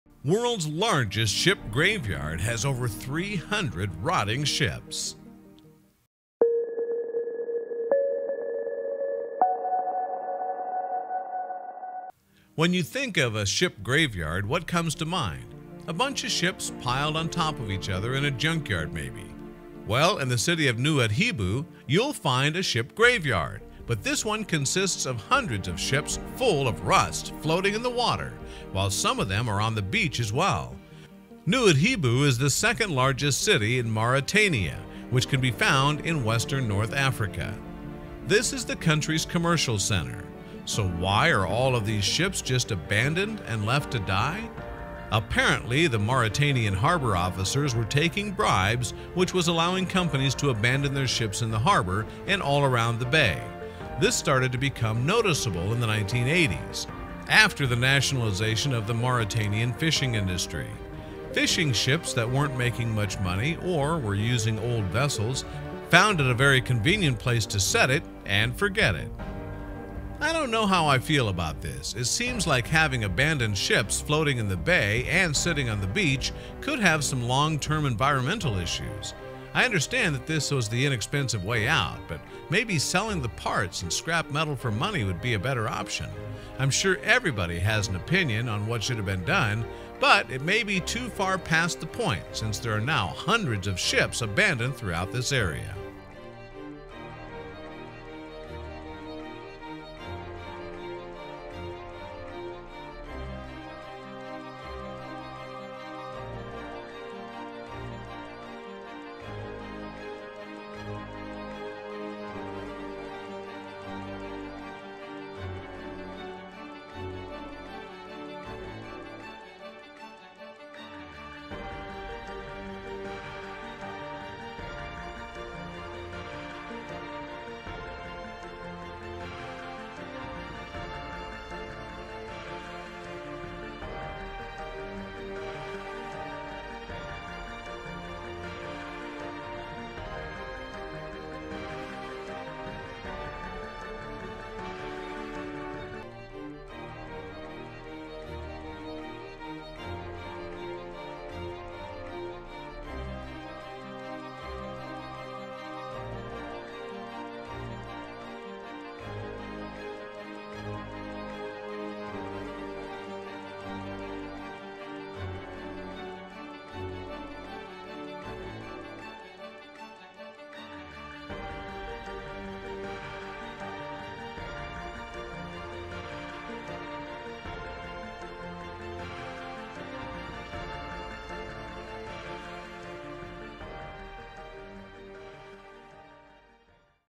Have You Seen The World’s Largest Ship Graveyard !
Extending from the west coast of Africa is Ras Nouadhibou, a small peninsula shared by Mauritania and Western Sahara. The east side of the peninsula belongs to Mauritania and is home to Nouadhibou, a city of nearly 100,000 residents and the second-largest settlement in the country. The region’s economic capital, Nouadhibou holds less illustrious titles as well: it is also home to the largest ship graveyard in the world.